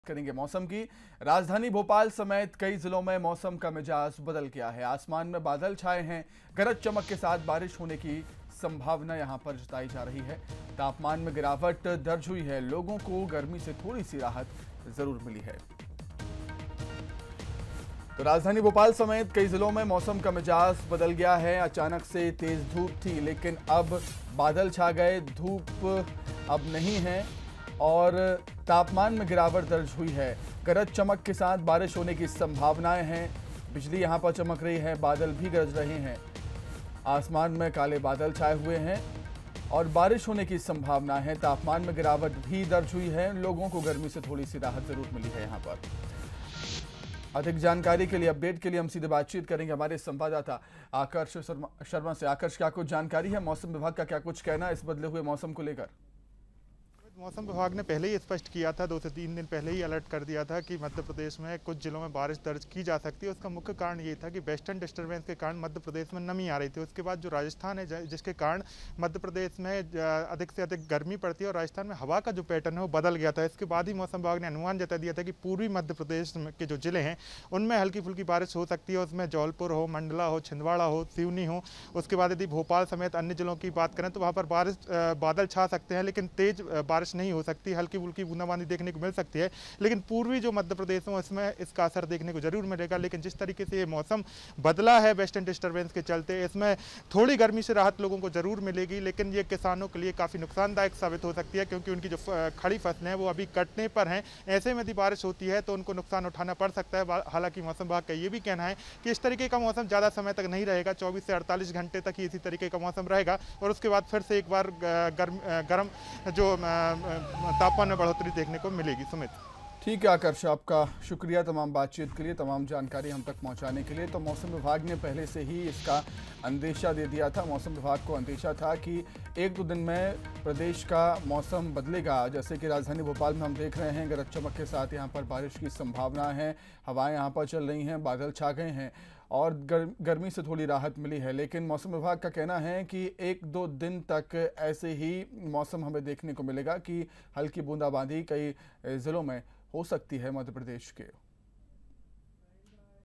Bhopal में बदला मौसम का मिजाज। मौसम विभाग ने जताई बारिश की संभावना
आसमान में छाए बादल, गरज-चमक के साथ बारिश होने की संभावना
तापमान में गिरावट, लोगों को मिली गर्मी से राहत
Madhya Pradesh Tempreture, Pachmarhi, Pachmarhi,  IMD Bhopal, IMD Gwalior, Weather Update , Weather Alert
, BansalNews ,Bansal News MPCG ,Breaking News,MP Breaking News,mp news in hindi, MP CG top news